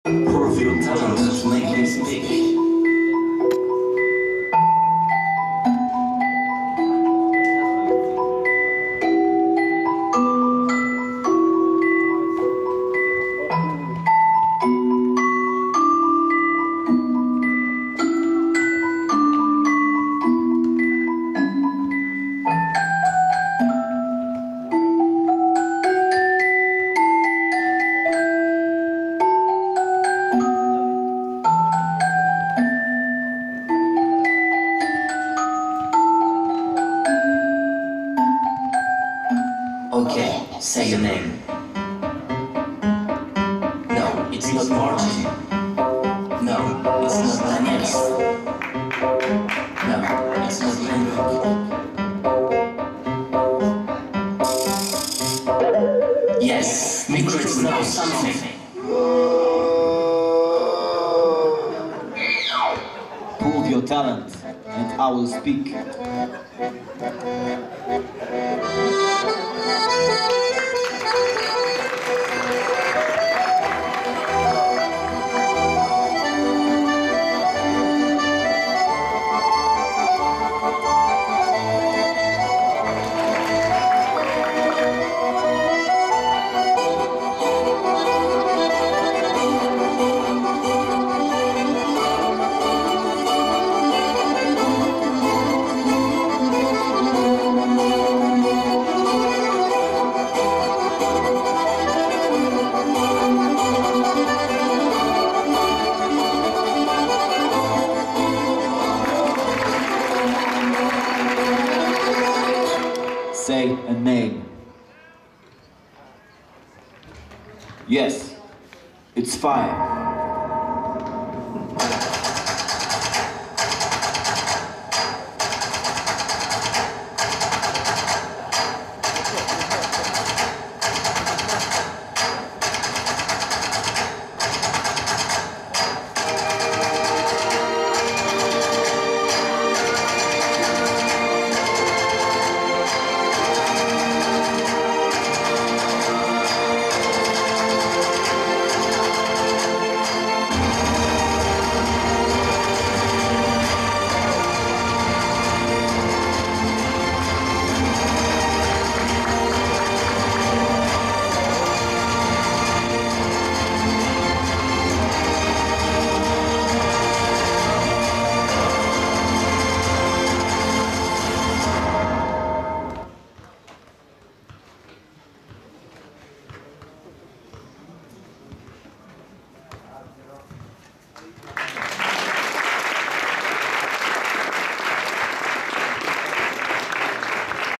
Gautier routine superball 2013 prague
Gautier's final routine. The winner of the routine competition